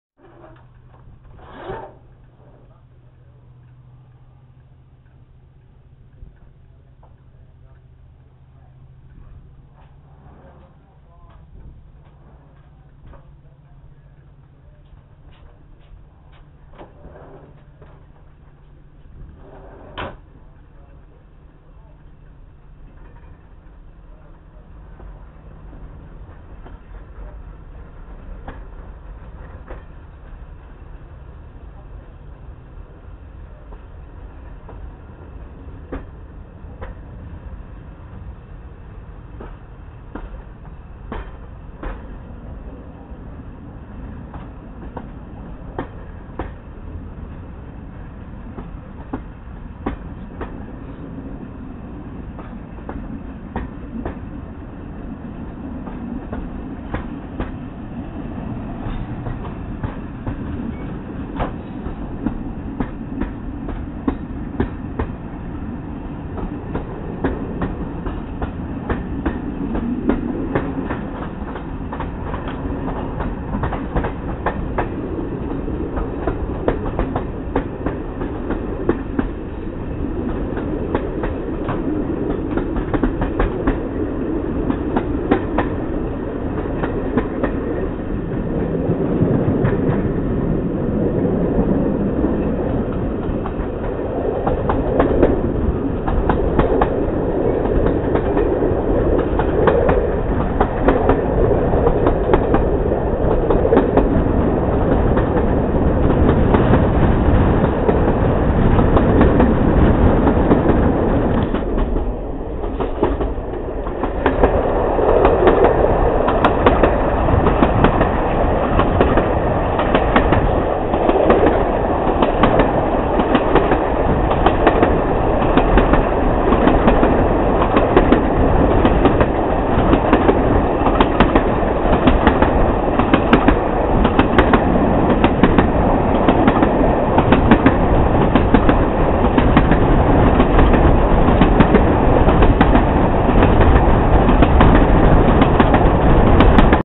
Far North Windowhanging -11- 55022 Georgemas Jc to Halkirk Block Post
Uninterrupted windowhang from Georgemas Junction to Halkirk Block Post.  Filmed on the SRPS Far North Challenger 17th and 18th of June 2011 from Linlithgow to Thurso and Wick hauled by Class 55 Deltic 55022 (D9000) Royal Scots Grey.